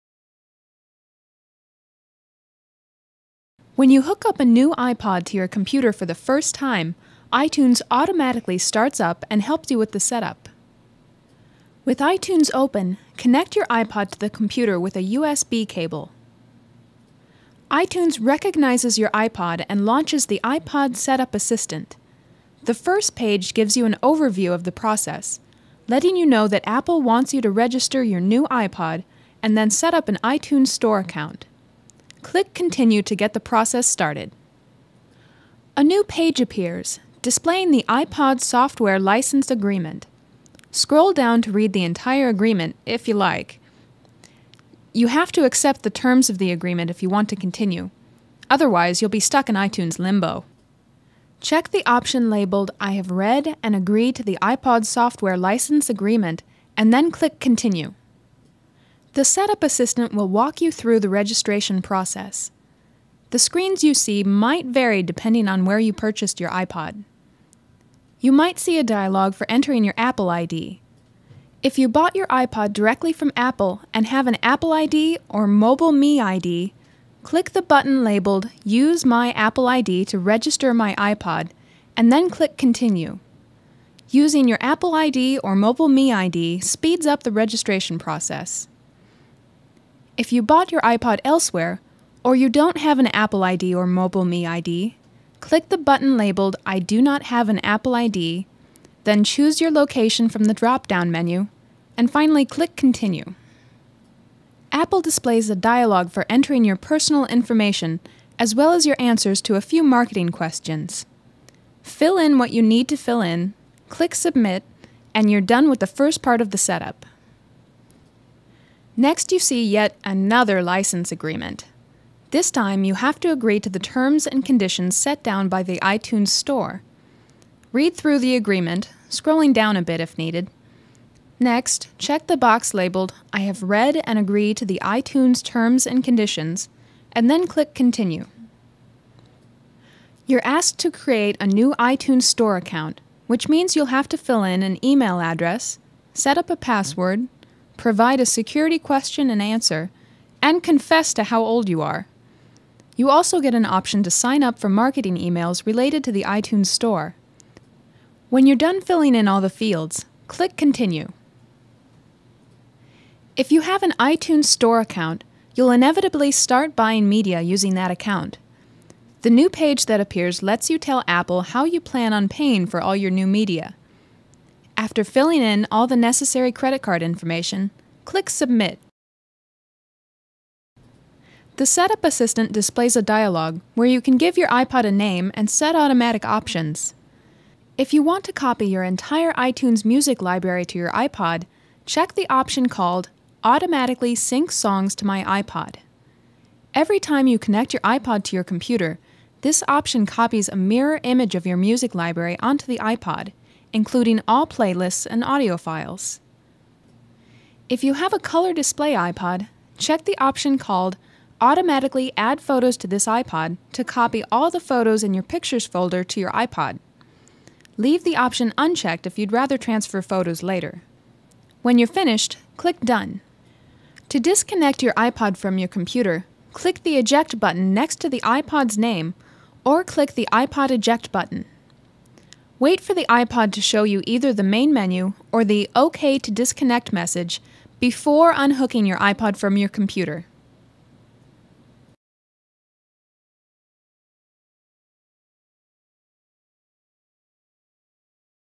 How to Set Up Your iPod For Dummies
Setting up an iPod Shuffle or iPod Nano is easy; iTunes helps after you plug in your USB cable. This video shows you how to set up and listen to music on your Apple iPod.